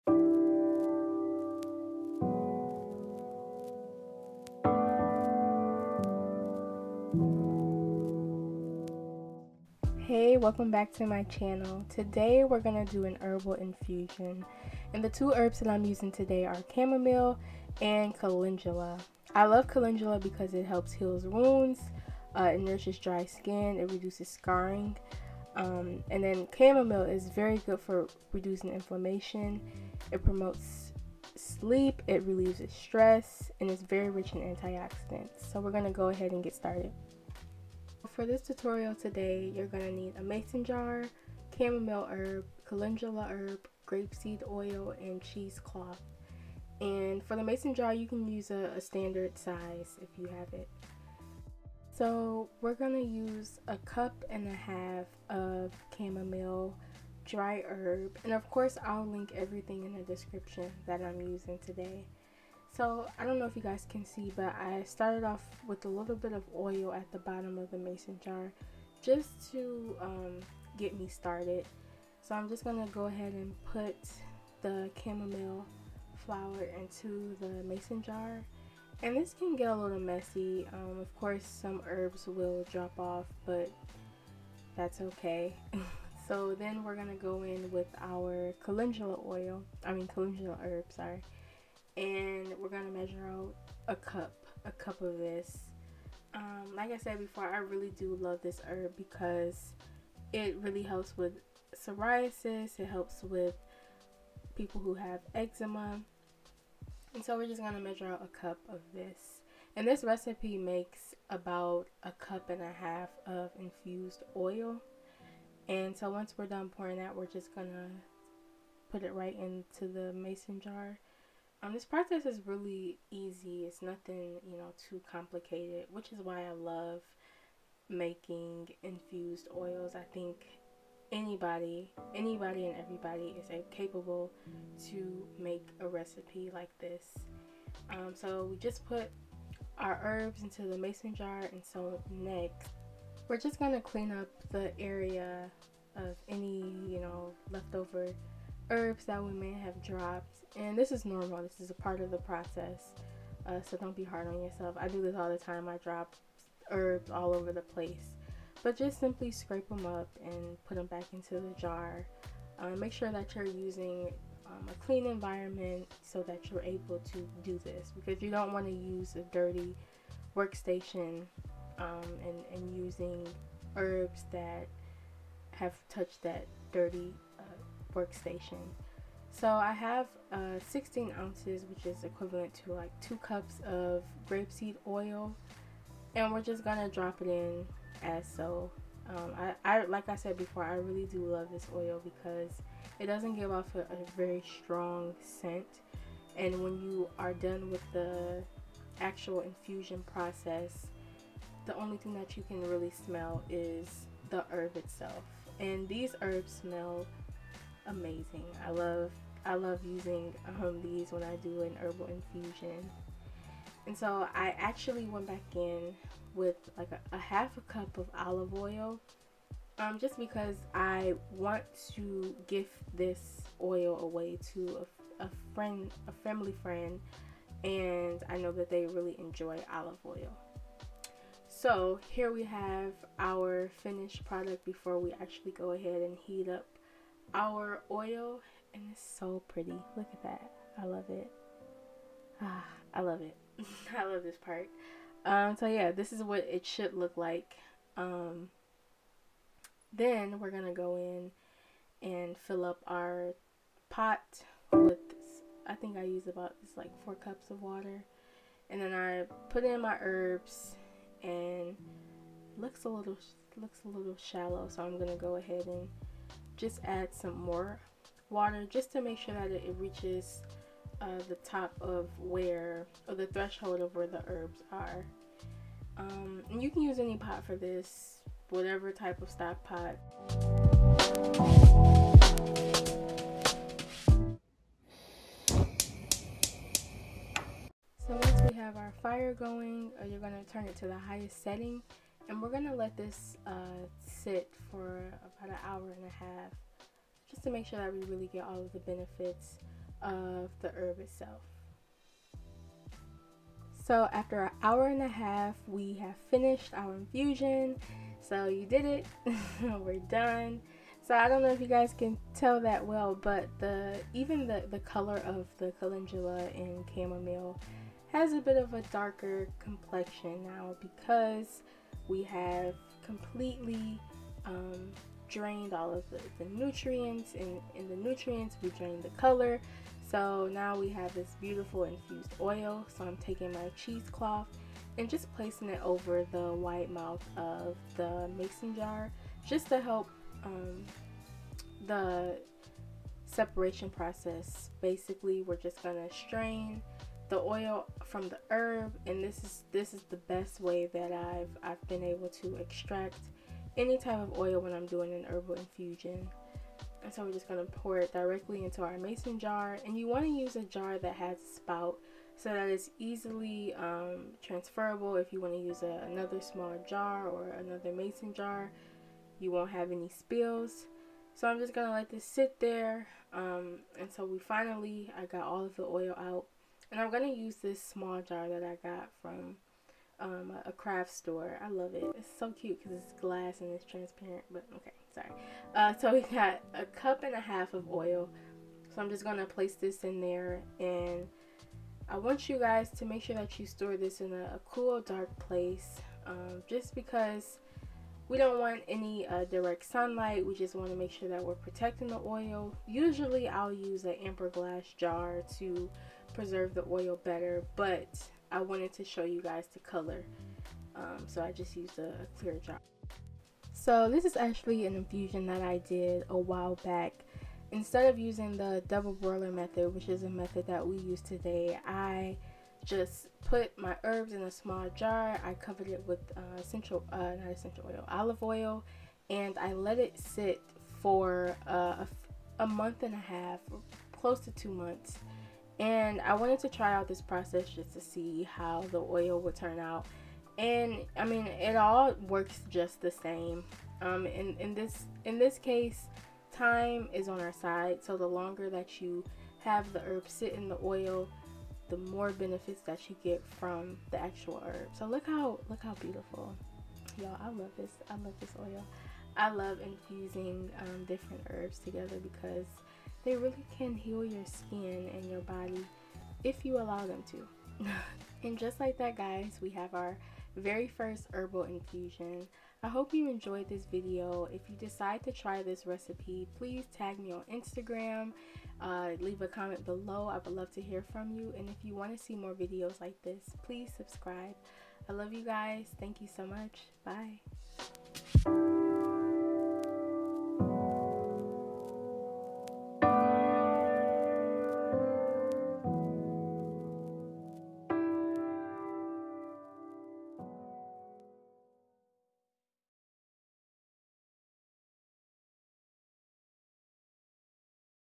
HOW TO MAKE AN HERBAL OIL INFUSION | DIY CALENDULA & CHAMOMILE OIL | SOOTHE YOUR SKIN
DIY Calendula and chamomile herbal infusion

In this video I teach you how to make your very own powerhouse herbal infusion oil. These herbs are known to help calm, soothe, and improve the quality of your skin. The best part is that you know each and every ingredient that is going into this infusion. You can use this oil all over your body to give your skin radiant glow. Anyone and everybody can use this oil because it is handcrafted with amazing herbs and love.

|| Benefits of each ingredient ||

Chamomile flower - The chamomile herb has so many powerful antioxidants that help regenerate skin and slows down the skin aging process. Blemishes and irritating redness can be lightened using chamomile. Anti-inflammatory herb.

Calendula flower - This herb is wonderful for problematic skin issues such as psoriasis and eczema. This medicinal herb has anti-fungal and anti-inflammatory properties that help heal wounds and scars.

Grapeseed oil - This beautiful carrier oil has the ability to even out your skin tone and improve hyperpigmentation of the skin. Grapeseed is a very lightweight oil that will surely make your skin glow.

|| Ingredients ||

1 ½ cup chamomile herb
1 cup calendula herb
2 cup grapeseed oil
½ cup olive oil
Cheese cloth
Mason jar (32 oz or 64 oz)

Ball Mason Jar
NOW Grapeseed oil
Calendula dry herb

|| Connect with M E ||

Listen to my podcast on Anchor: Tia Tales All